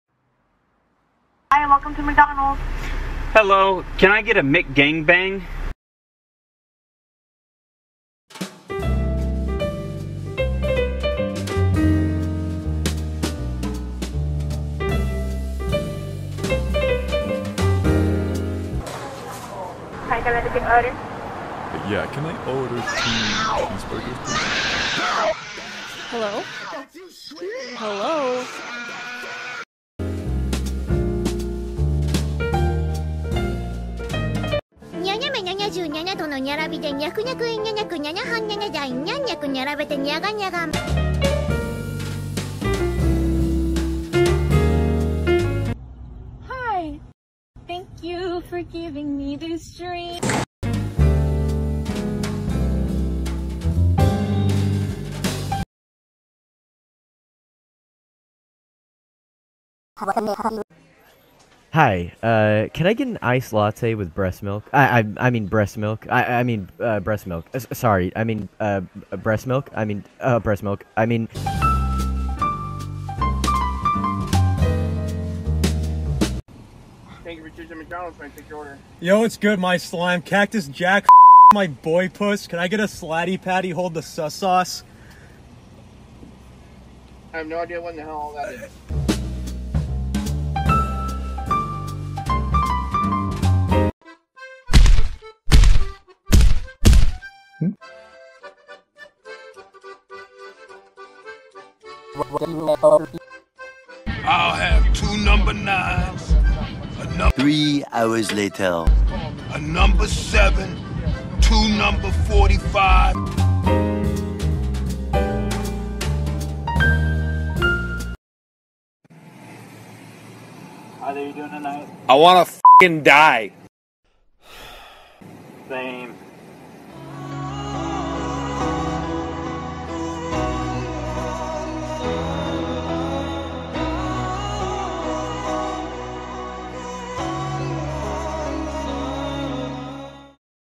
POV: You don't get paid enough.
This is an animation conveying some of the shenanigans that go on in the fast food industry.

Hi, I'm cynicalbash2 and I make silly and random animations. :)